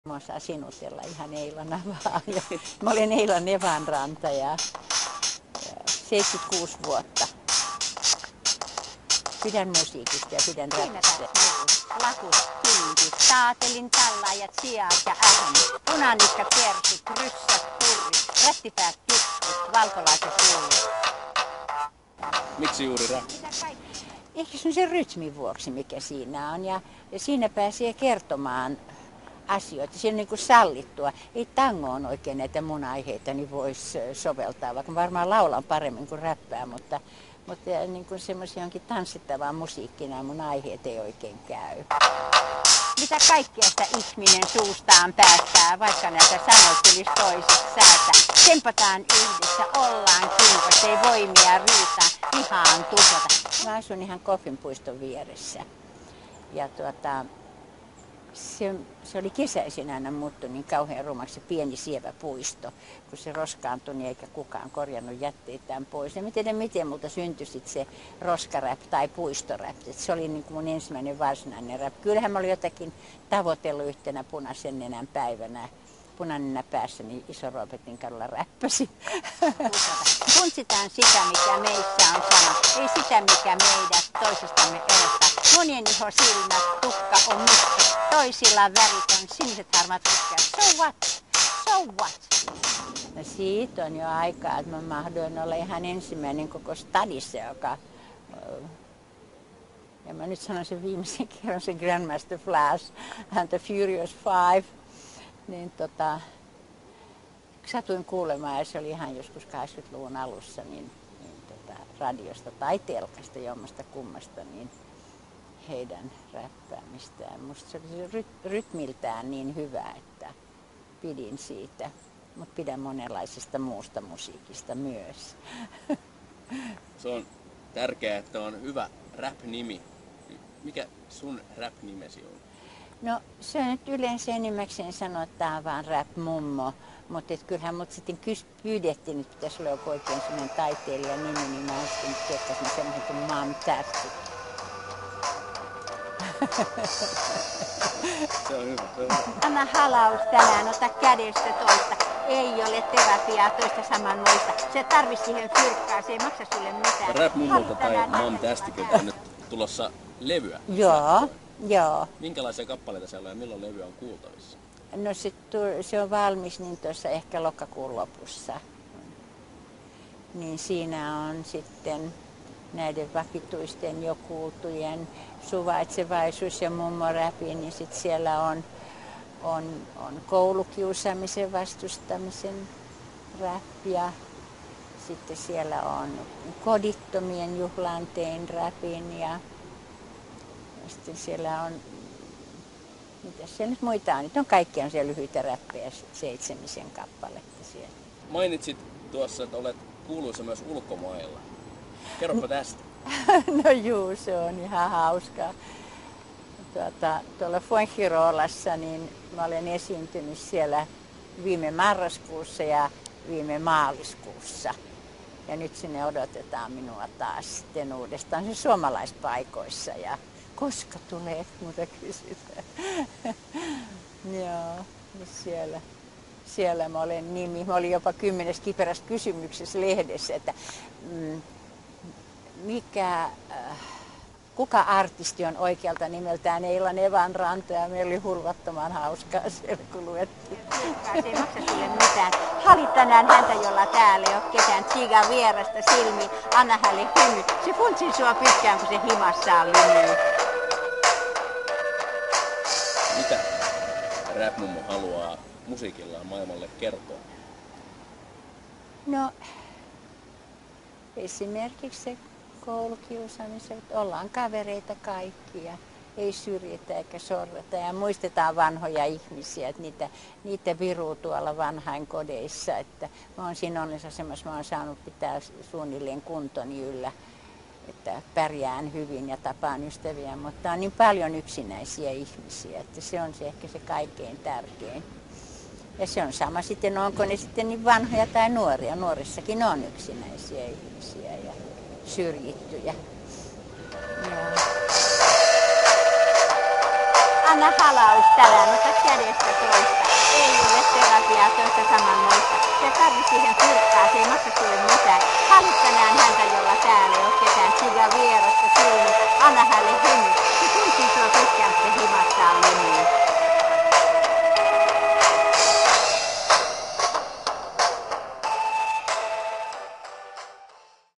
Rap-mummo säväyttää sanoituksillaan! 2012
Rap-mummo is bäck.
Eila Nevanranta on harvinainen 76-vuotias nainen. Vanhuusiän tylsyyttä vastaan hän taistelee matkustelun ja ulkoilun lisäksi tekemällä musiikkia. Eikä mitä tahansa musiikkia, vaan rap-musiikkia. Kuuntele Eilan tarina.